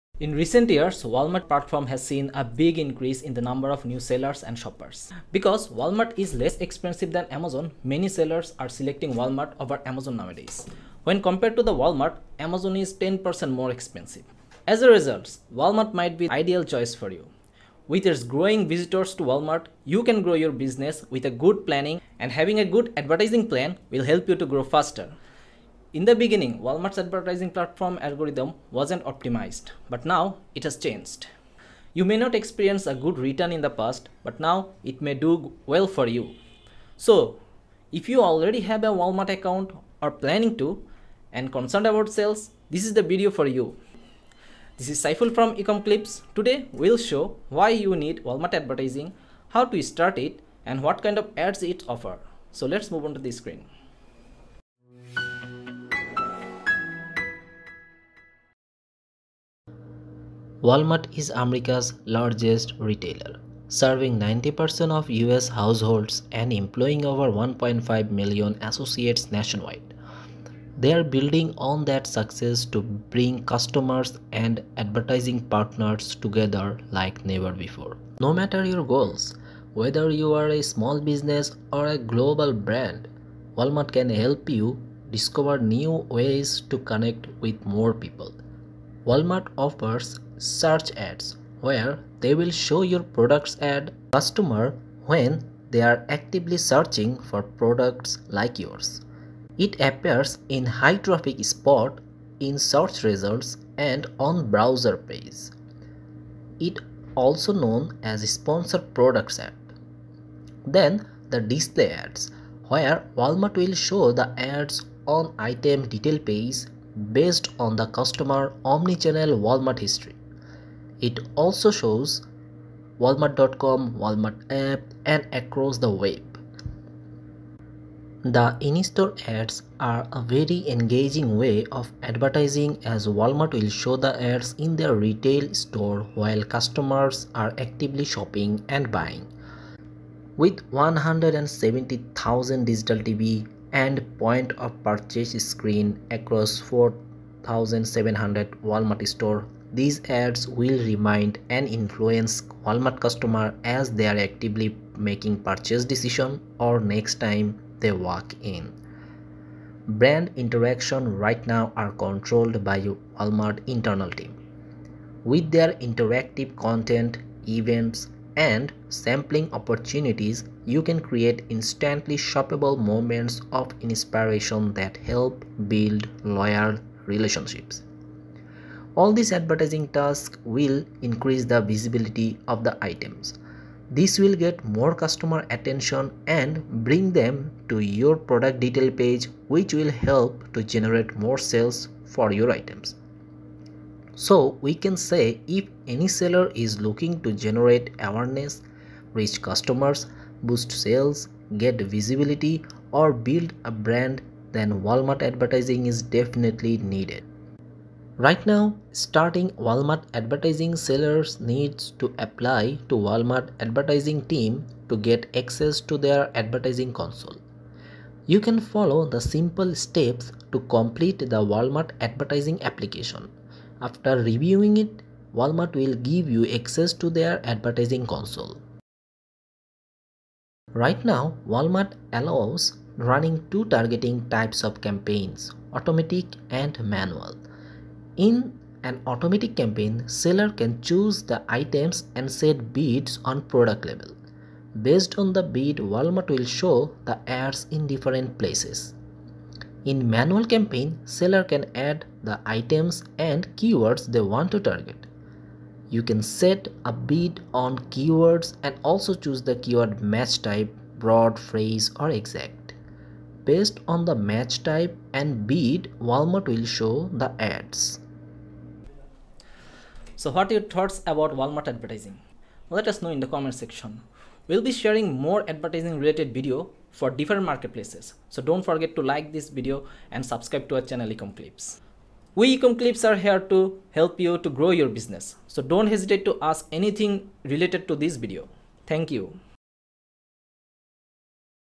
Walmart: Why Need Walmart Advertising, How to apply for Advertising Console, Ads Offer by Walmart
Walmart is one of the largest retailers in the world. As Walmart continues to build out its marketplace capabilities, the number of approved sellers has been climbing at an increasing pace. And if you want to keep up with this pace you will need to have a good advertising plan in order to succeed. If you sell on Walmart and want to reach more eCommerce shoppers, you need to be advertising through Walmart. Plus, advertising on Walmart can help you reach more in-store customers too.

Walmart ads can help to show ads in the below format.

✅ Search
✅ Display
✅ In-store
✅ Brand interactions

So if you are looking to generate awareness, Reach customers, Boost sales, Get visibility, or Build a Brand then Walmart advertising is definitely needed.

✅ Company name
✅ Number of SKUs (offered by your business on Walmart.com)
✅ Product category (the primary category of products sold by your business)
✅ Primary campaign objective
✅ Target audience
✅ Desired advertising plan (annual, seasonal, etc.)
✅ Anticipated budget

In Walmart, Campaigns are broken down into two targeting types automatic and manual. Like Amazon, Walmart also offers 3 match types in their manual campaigns. In automatic campaigns, they still didn't introduce the negative keywords adding option. In automatic campaigns, you can bid on product level.

Automatic Campaigns: Auto campaigns match your ads with consumer searches based on relevancy, which is determined by product listing information and bid. You can bid on a product level in an automatic campaign.

Manual Campaigns: Manual campaigns allow you to control which search terms you bid on by entering keywords into those campaigns. Also, let you pick the keywords to match types.

If you require any additional assistance, please leave a comment in the comment section, and we will get back to you with the proper solution.

Looking forward to seeing you in the next video.